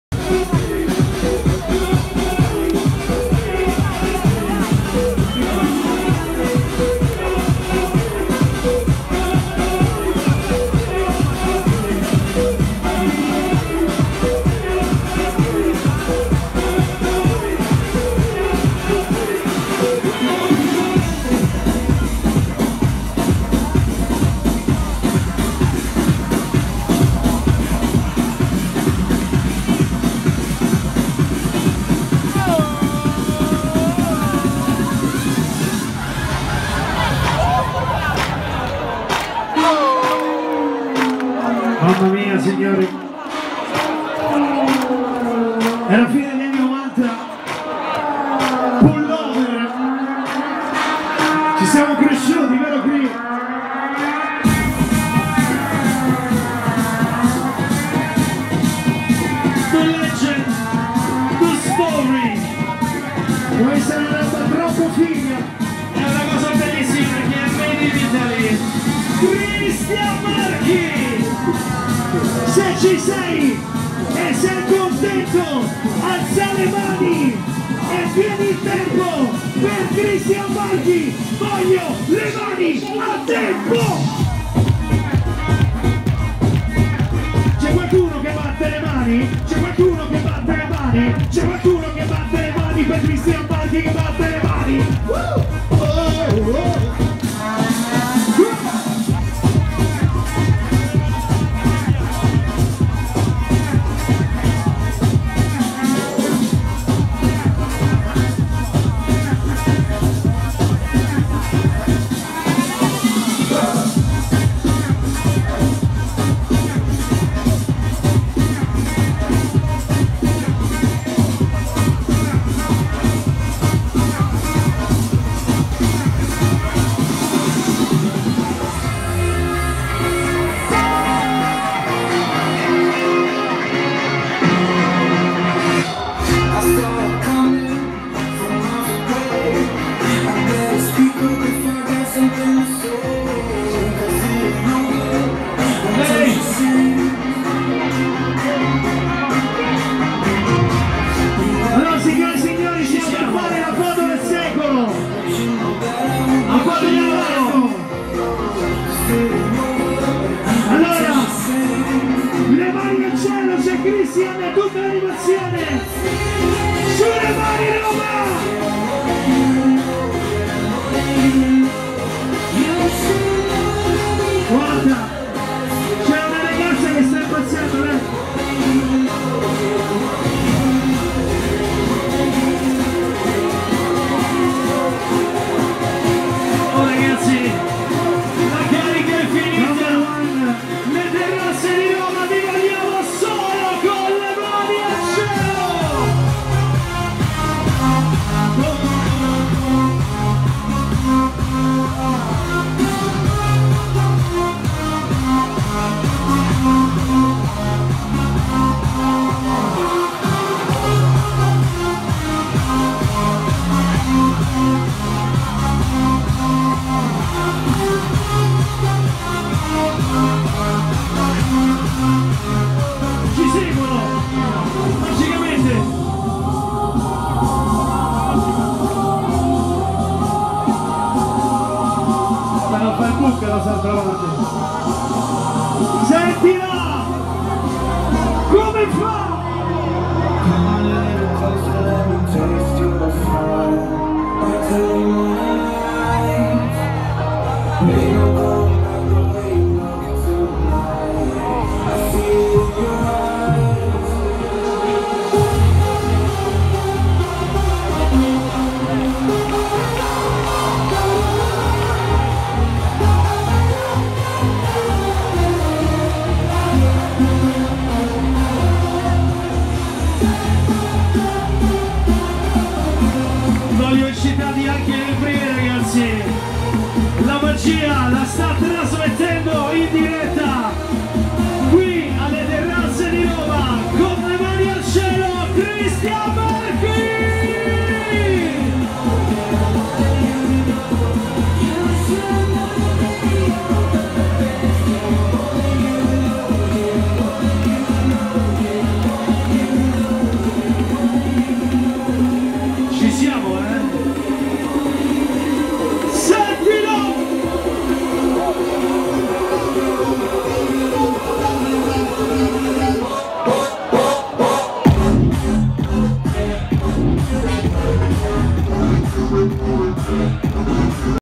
CRISTIAN MARCHI apre le le danze alle Terrazze Lupi Solitari Eventi Roma Diskoteka.it
CRISTIAN MARCHI apre le le danze alle Terrazze di Roma 22/06/2018
By Riccardo Del Zio